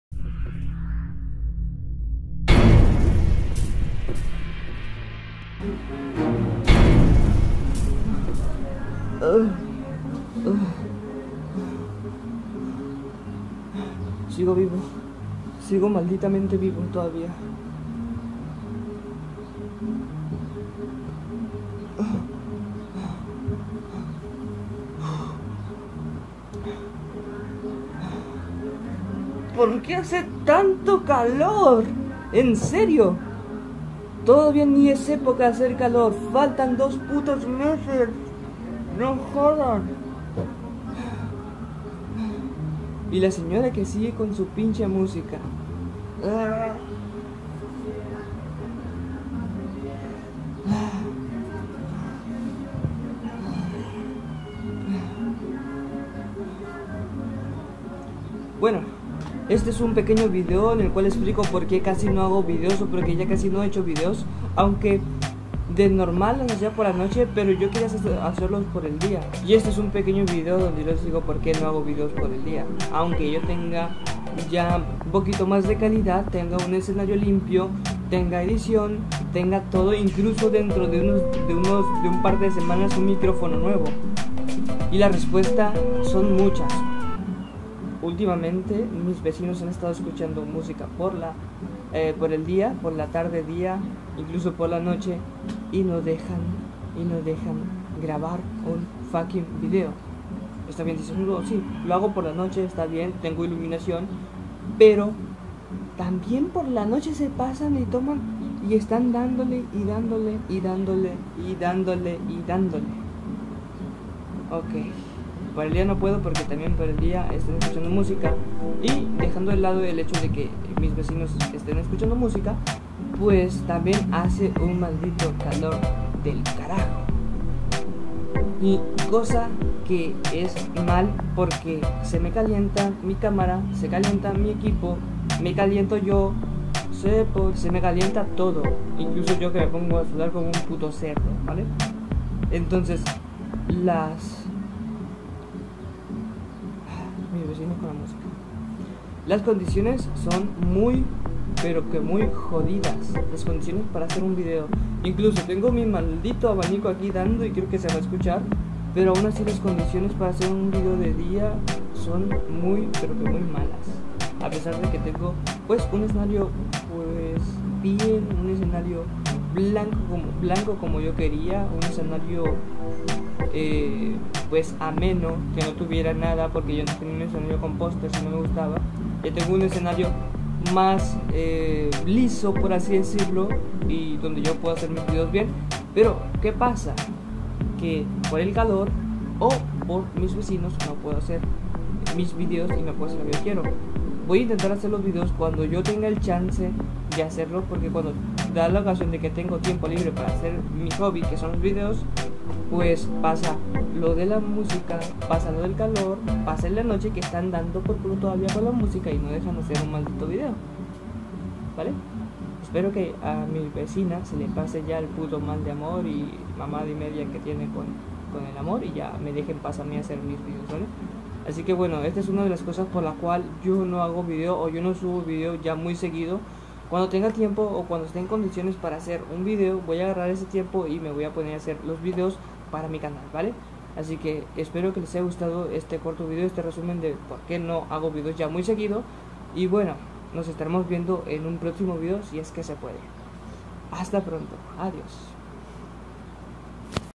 RAZON POR LA QUE NO SUBO VIDEOS
Tengo tiempo sin estar subiendo seguido videos, y esta es la razon por la que casi no subo.